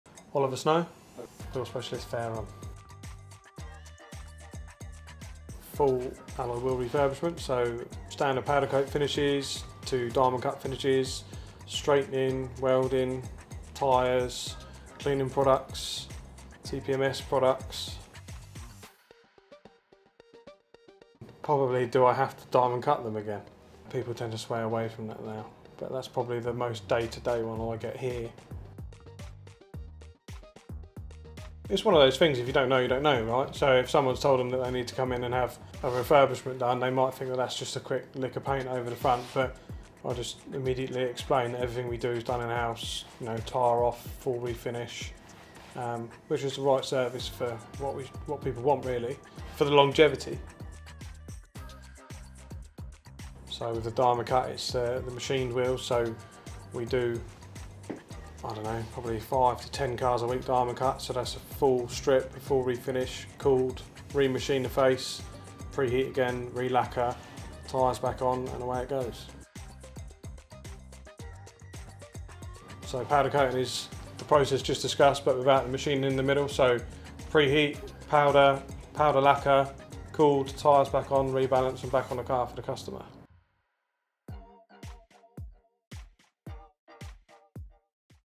Ask The Wheel Specialist a Question
Olly Snow from our Fareham branch is here to answer some of the questions we commonly get asked.

Have you got a burning question about wheel refurbishment?

Let us know, and we will try answering in a future video!